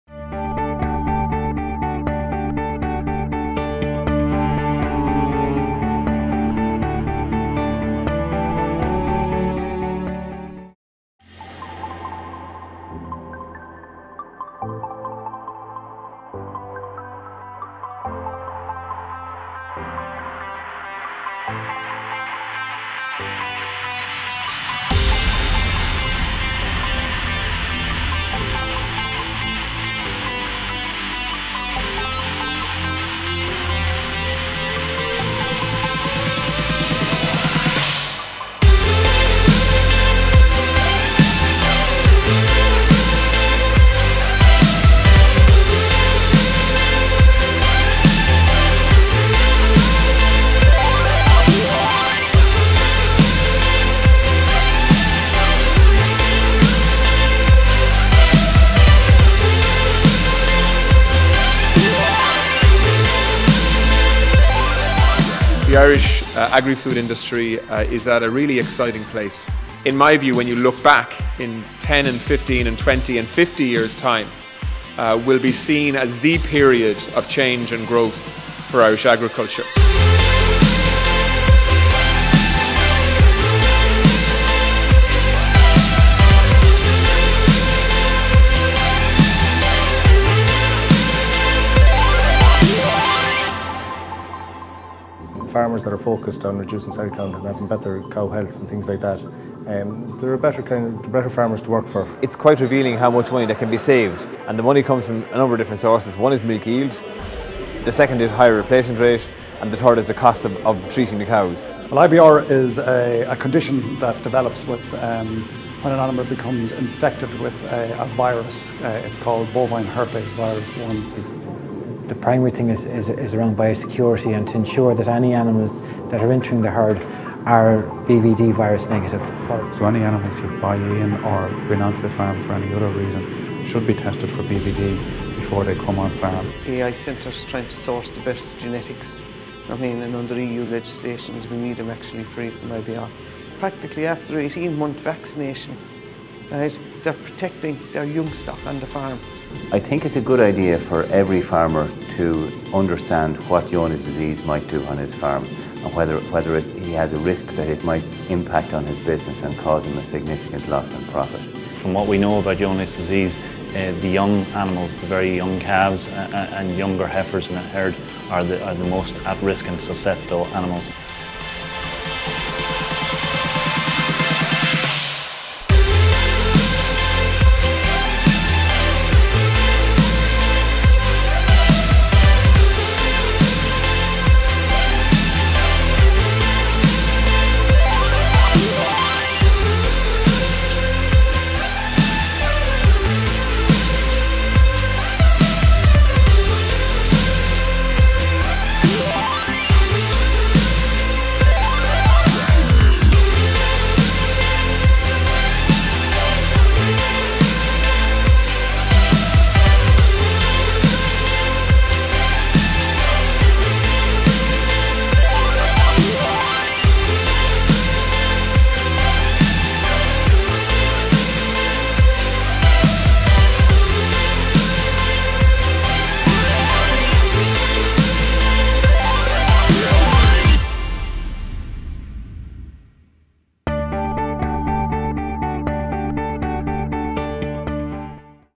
Animal Health Ireland Conference 2013
dairymaster buildingbetterproducts The highlights of the 2013 Animal Health Ireland Conference which took place in Rochestown Park Hotel, Cork, Tuesday 22 to Wednesday 23 October 2013.

A major focus of the conference was the Cell Check Program which centres around the health and financial benefits of mastitis control in farming. The 2013 AHI Conference also focused on IBR, BVD and Johne's Disease.

This video was kindly produced by the Dairymaster Digital Media Team.